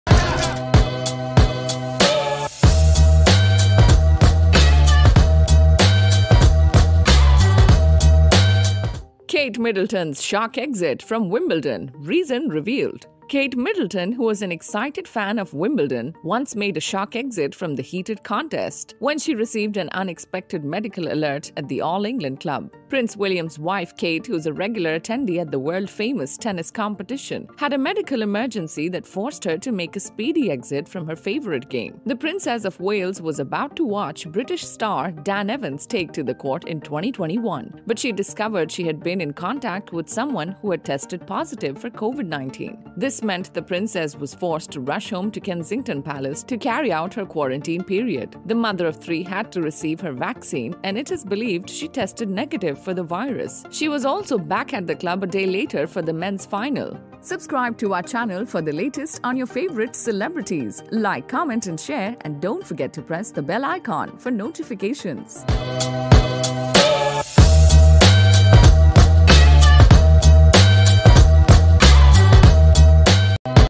Kate Middleton made shock exit from Wimbledon after medical alert
Kate Middleton, who's an excited fan of Wimbledon, once made a shock exit from the heated contest when she received an unexpected medical alert at the All England Club.